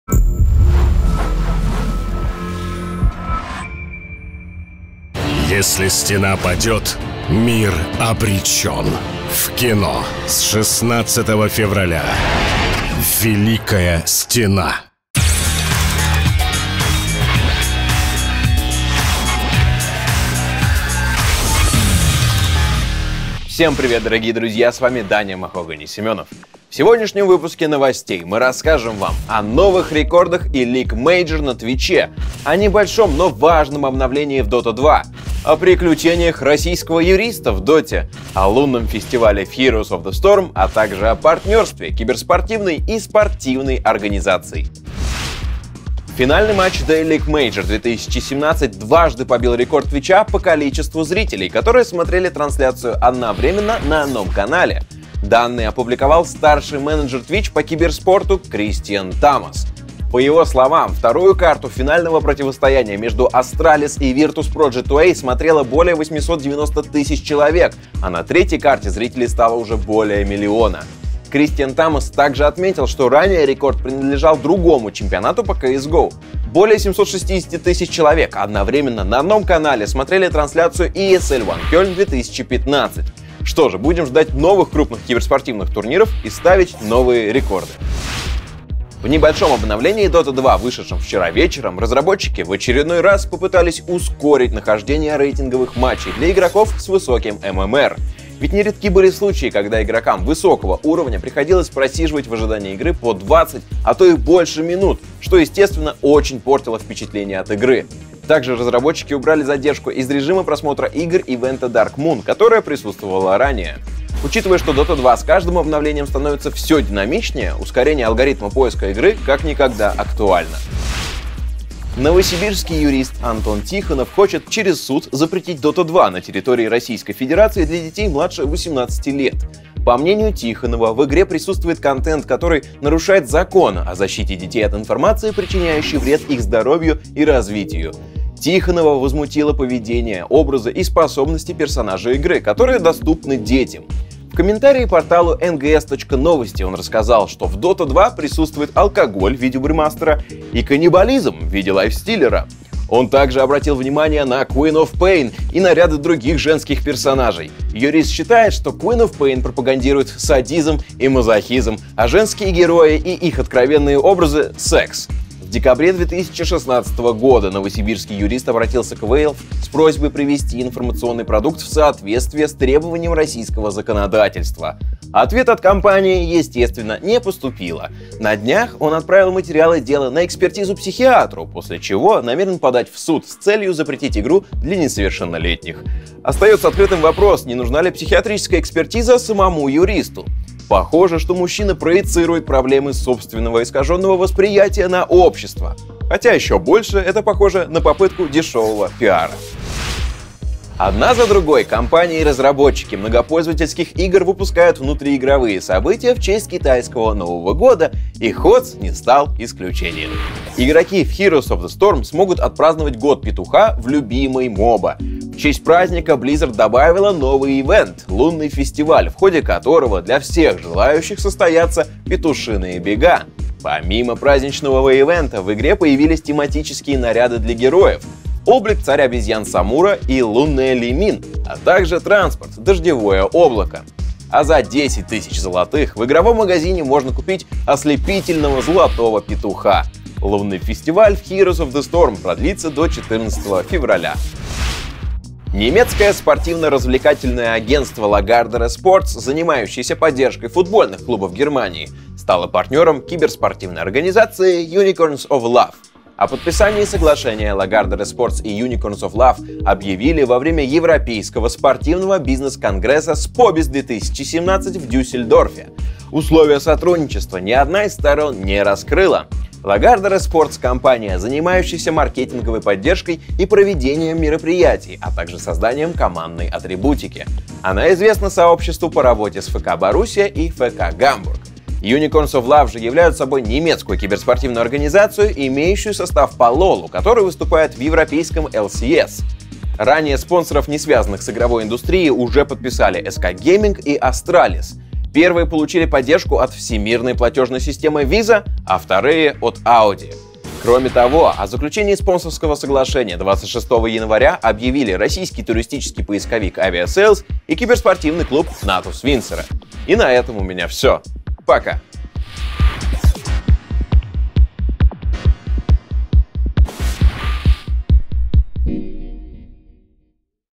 Новости: Возможный запрет Dota2 в России и два рекорда ELEAGUE Major
ELEAGUE Major дважды побил рекорд Twitch по числу зрителей; нахождение игры на высоком MMR в Dota2 стало быстрее; Российский юрист решил защитить детей от Dota2; Лунный фестиваль в HotS; и заключение партнерства спортивного агенства Lagardere Sports с организацией Unicorns of Love. Смотрите подробности в этом выпуске.